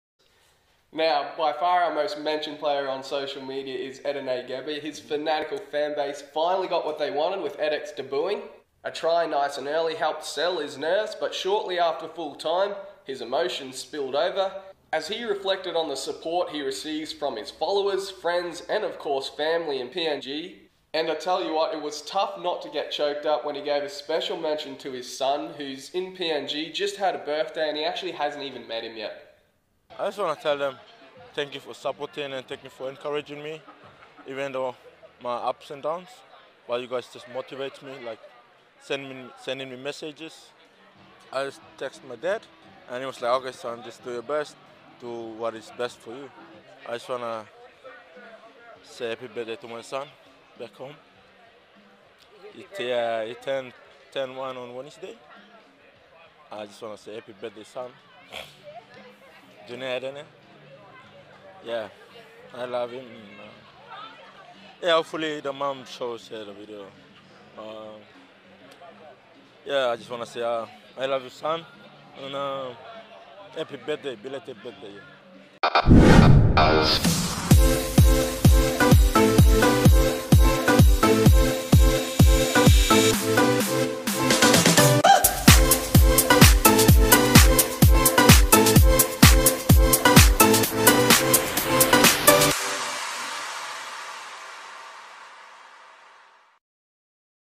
Edene Gebbies long awaited debut for Townsville Blackhawks.
Edene Gebbies Debut for Townsville Blackhawks. Postmatch emotional Shout-out to his son in PNG.